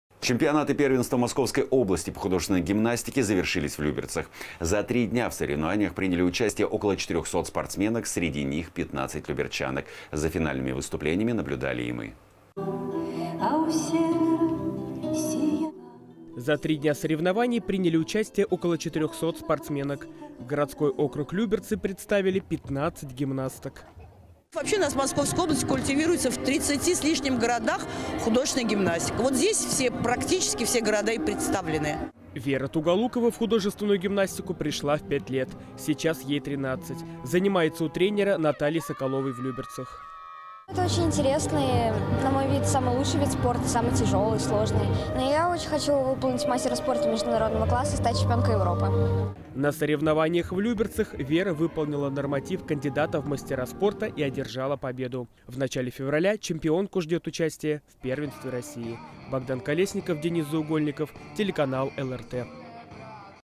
Покорили пластикой и грацией
Чемпионат и первенство Московской области по художественной гимнастике завершились во Дворце спорта «Триумф». Среди победителей – спортсменка из Люберец. Подробнее – в нашем следующем сюжете.

За три дня в соревнованиях приняли участие около 400 спортсменок. Городской округ Люберцы представили 15 гимнасток.

Елена Розанова, вице-президент Федерации художественной гимнастики Московской области:

«Вообще у нас в Московской области культивируется в 30 с лишним городах художественная гимнастика. Вот здесь практически все города и представлены».

Вера Туголукова в художественную гимнастику пришла в 5 лет, сейчас ей 13. Занимается у тренера Натальи Соколовой в Люберцах.

Вера Туголукова, участница первенства Московской области по художественной гимнастике:

«Это очень интересный, на мой взгляд – лучший вид спорта, самый тяжёлый и сложный. Я очень хочу выполнить мастера спорта международного класса и стать чемпионкой Европы».

На соревнованиях в Люберцах Вера выполнила норматив кандидата в мастера спорта и одержала победу. В начале февраля чемпионку ждёт участие в первенстве России.